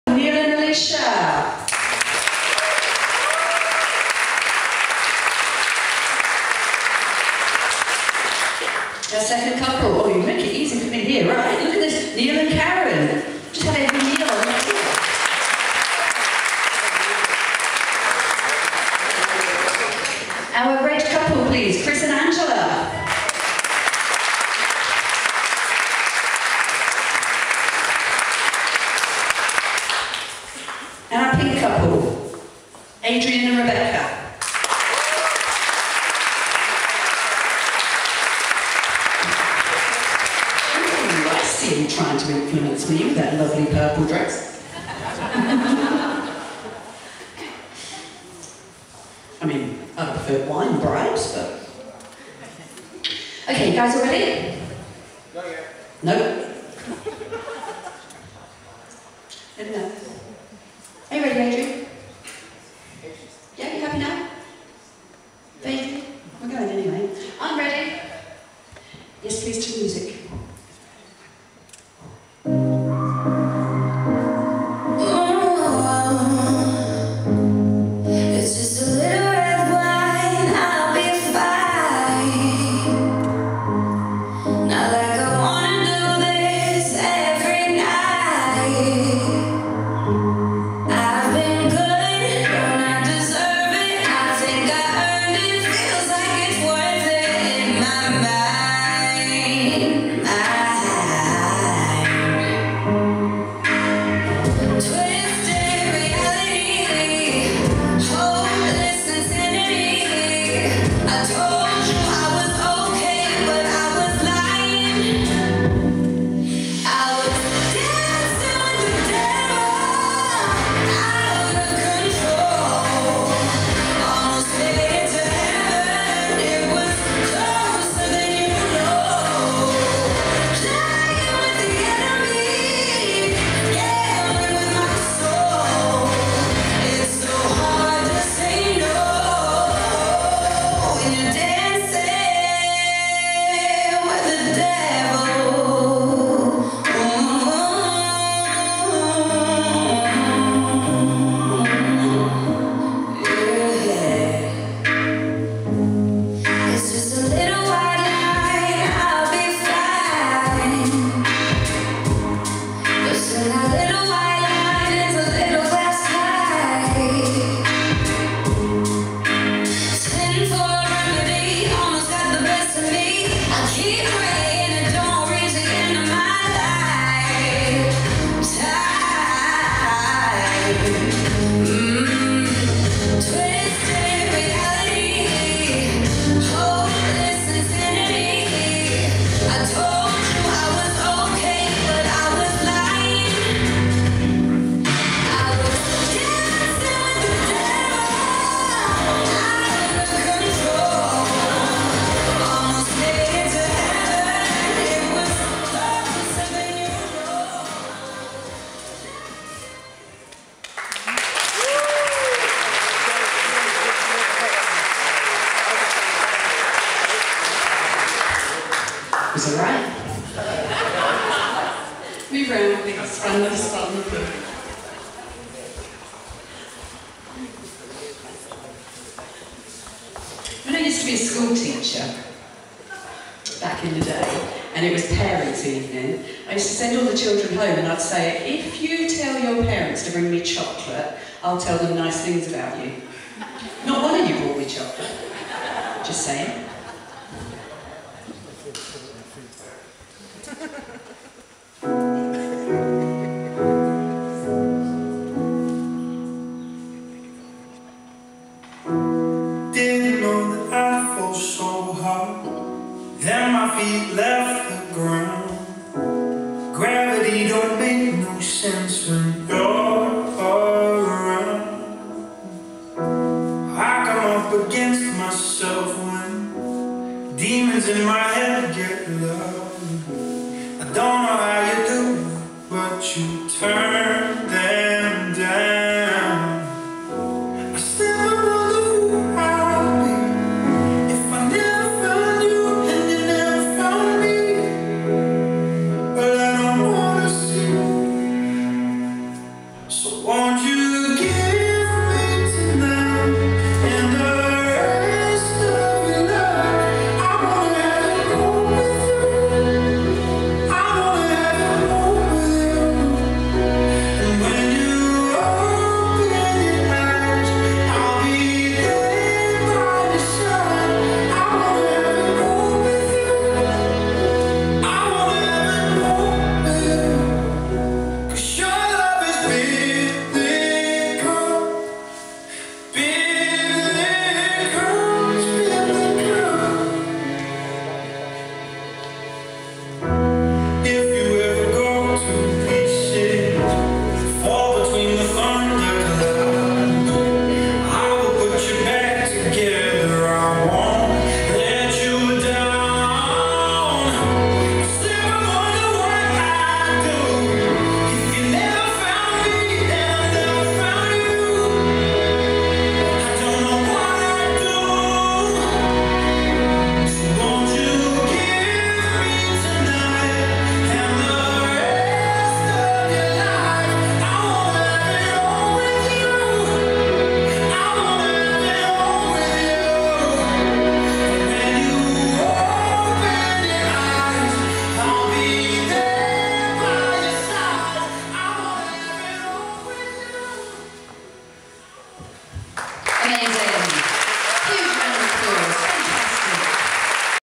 Crystal Blues 2022 - Dance Like Sara Watching - Heat 2
The 12th National Crystal Blues Challenge
Held in Buckden Village Hall October 2022
run by Sara White

Judges:
Rob
Alan
Maja
Craig
Paul
The whole audience get a collective vote also.

DJ's and Music choices by:
Alan, Myra, Morris, Sara, Graham

Front of house Team:
Tim, Debs, Rachel, Laura

Crystal Ladies:
Di, Lesley, Mary, Andrea

Kitchen Queens:
Niari, Tass, Grace

The competition was also an opportunity for attendees to donate to the Local food bank, with a large collection of food, clothing and items donated.